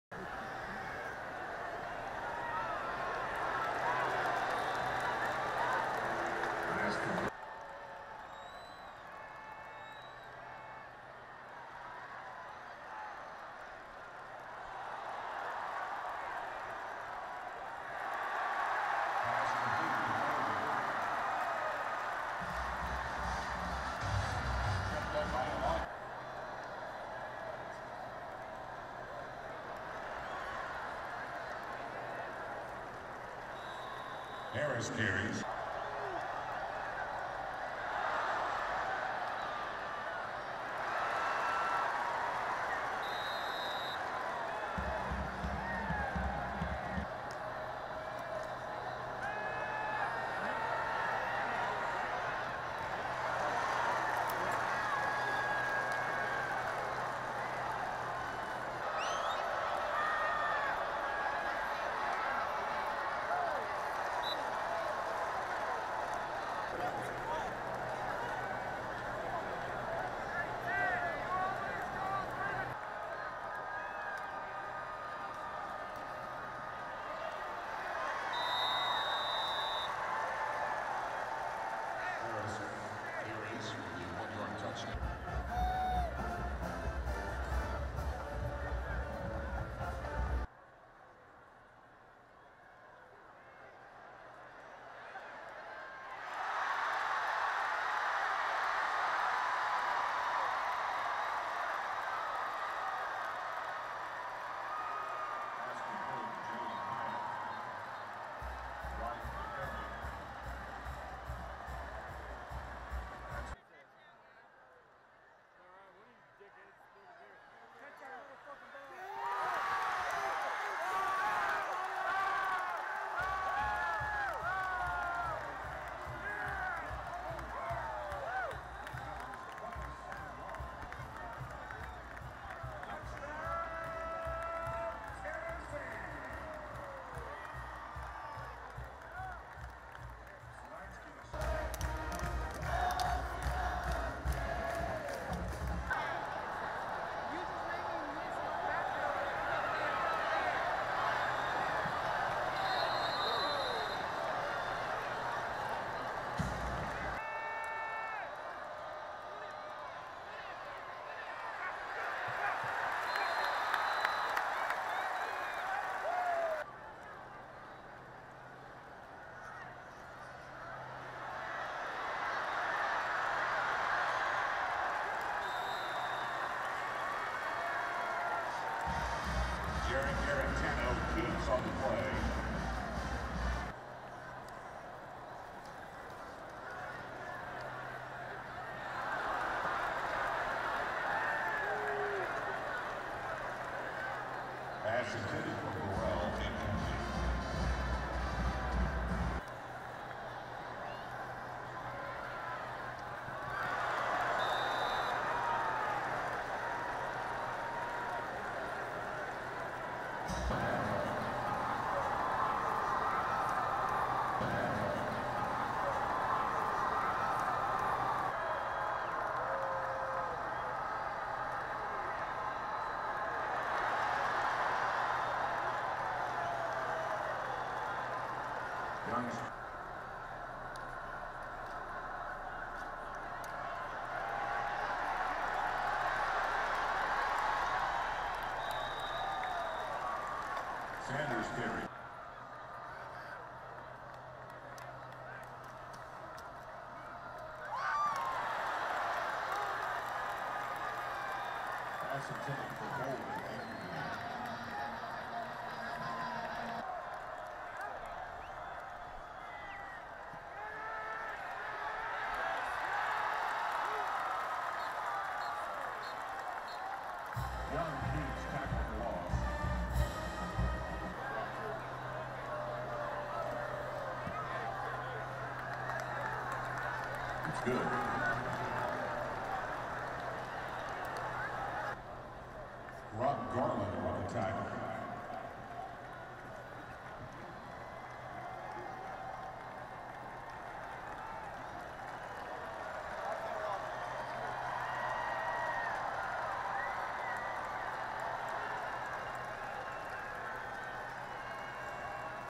Alabama at Tennessee Highlights - Field Level - 2nd Half
10-24-20. Courtesy of SEC Media Portal. More at 991TheSportsAnimal.com